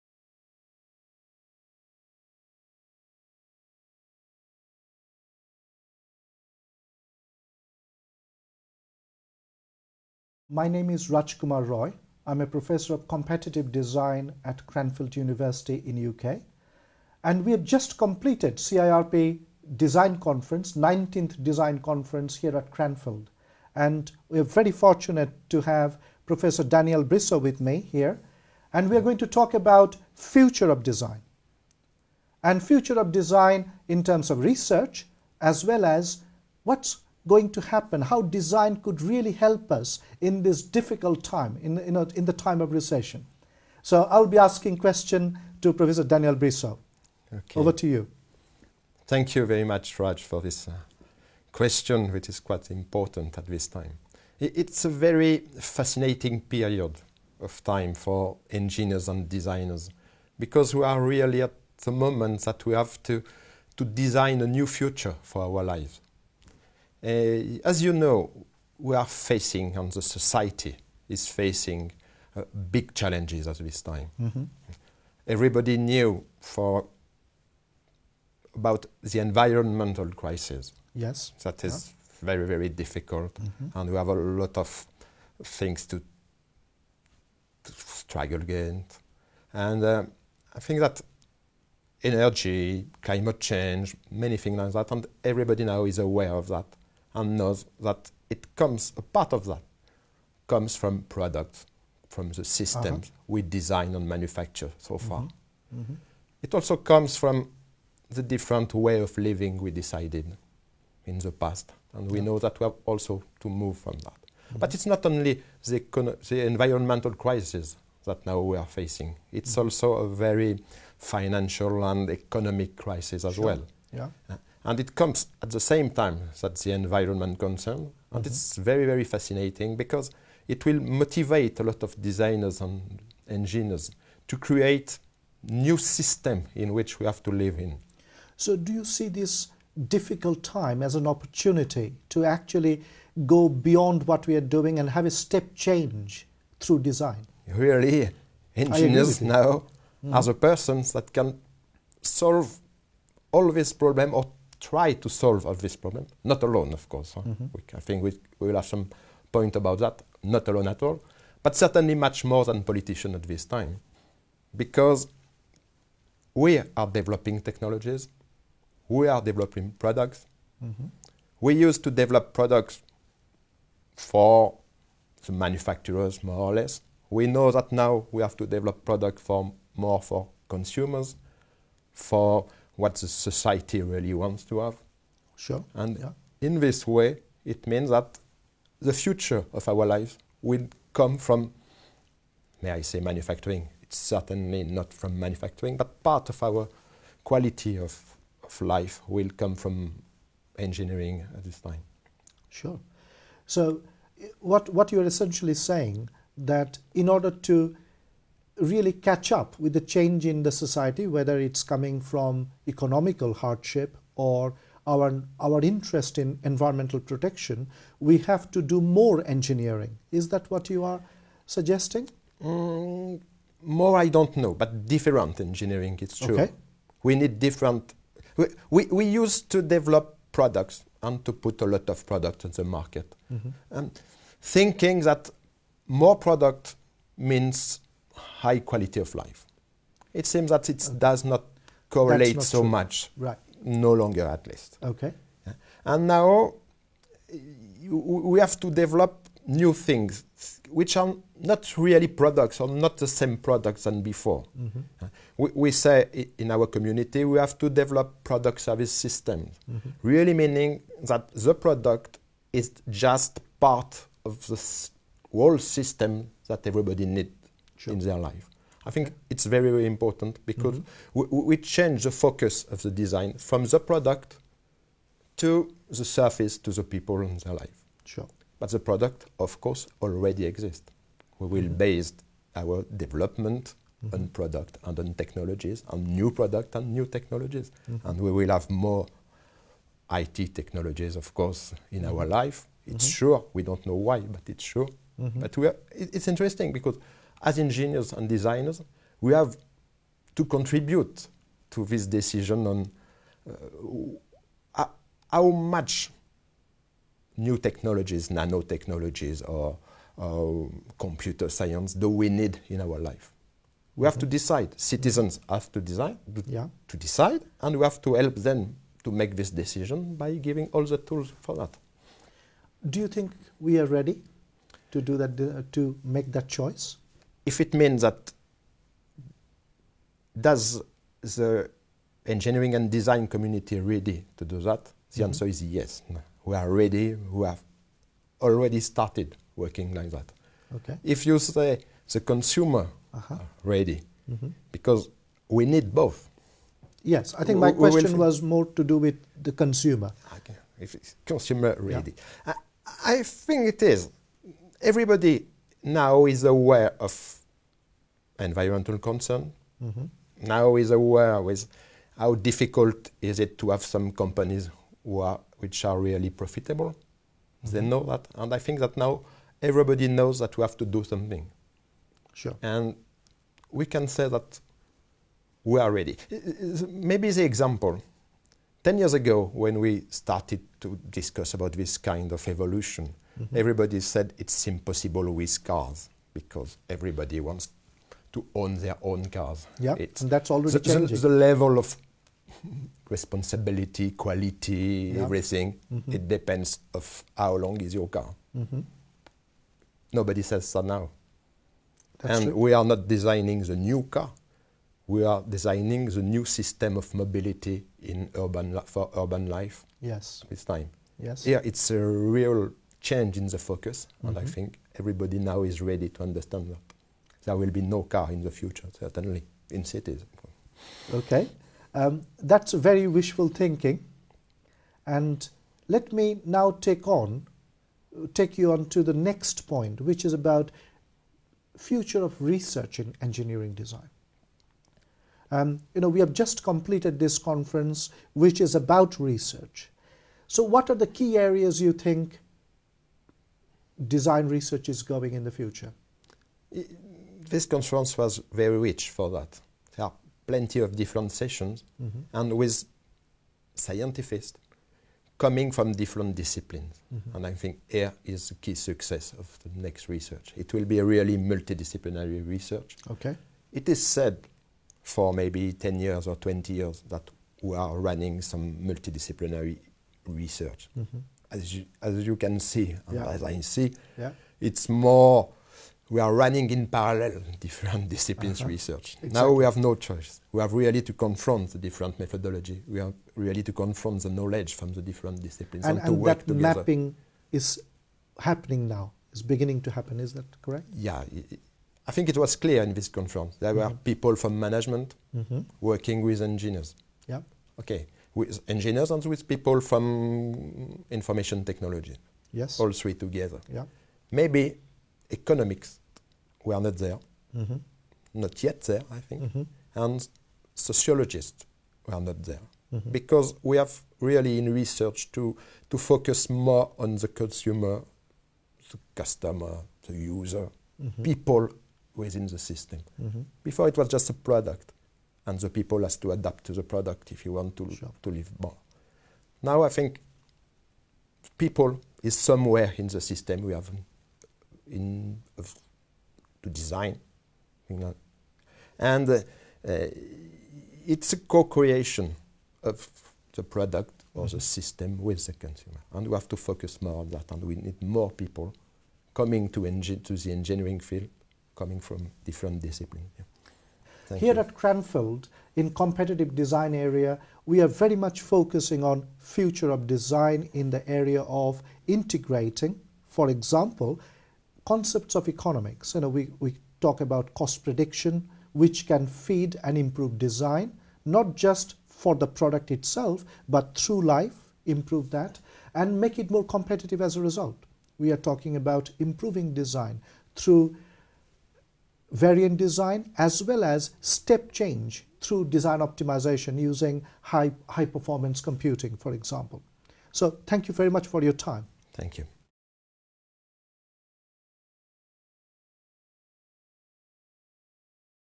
Competitive Design - Future of Design
Professor Daniel Brissaud (Professor of Engineering Design at INP Grenoble) speaks with Professor Rajkumar Roy (Professor of Competitive Design at Cranfield University). Recorded at the 19th CIRP Design Conference held at Cranfield University in March 2009.